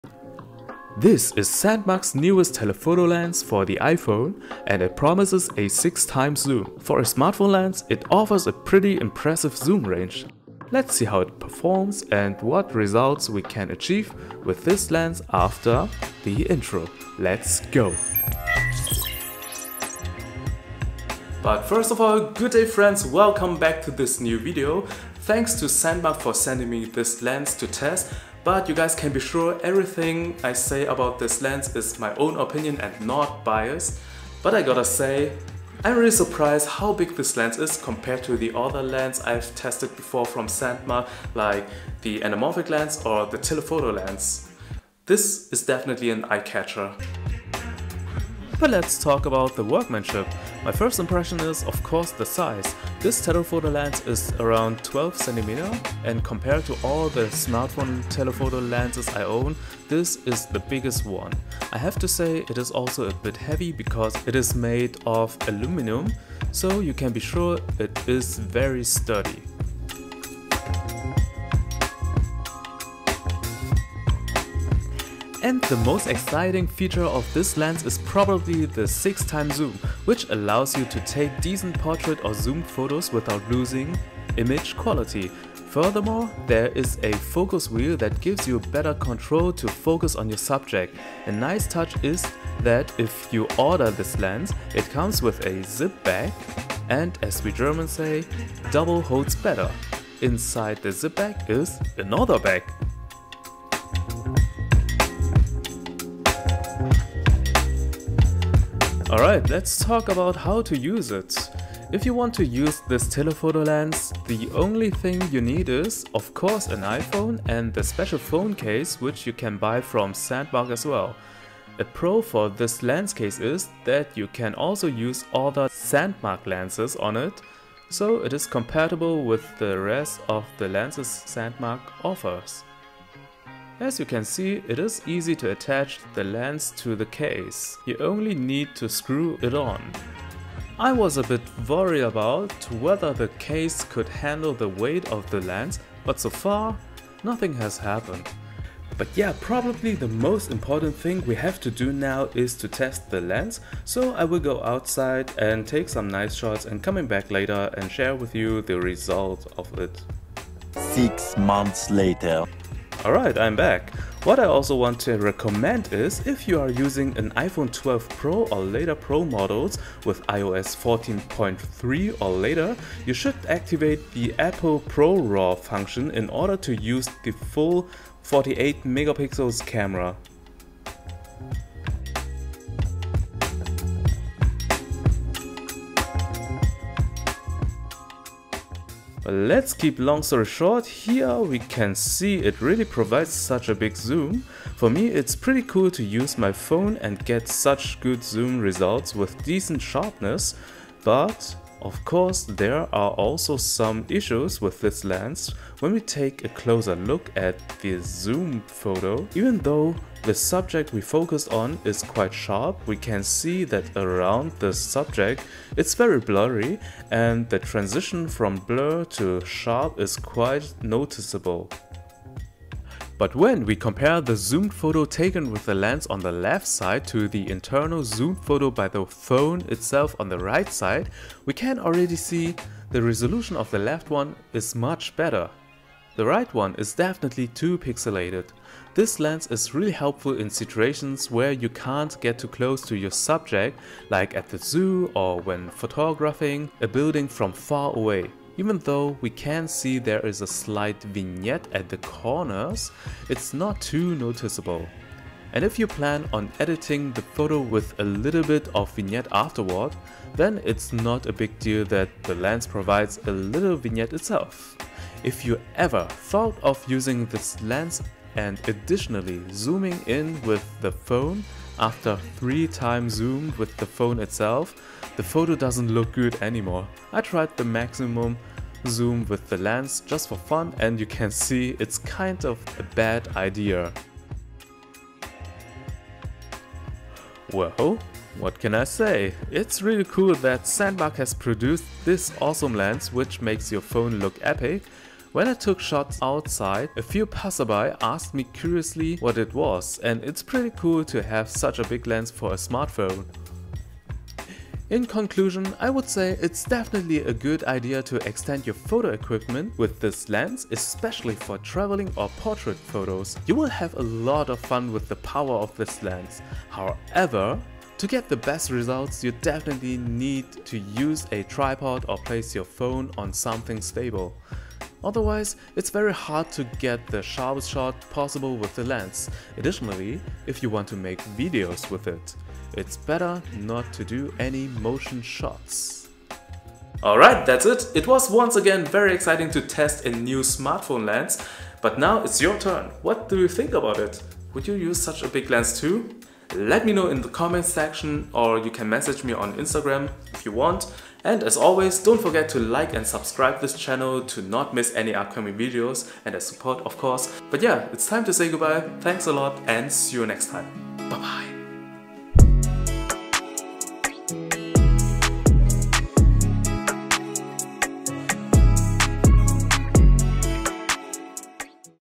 As good as it looks? 6x zoom Lens for iPhone
Hey Guys! I got the chance to make a review of this tele lens for iPhone from @Sandmarc, thanks a lot! It´was quite interesting to review and test this lens, in this video you can see some photo comparisons of the iPhone camera and the one with the tele lens.

Sandmarc Equipment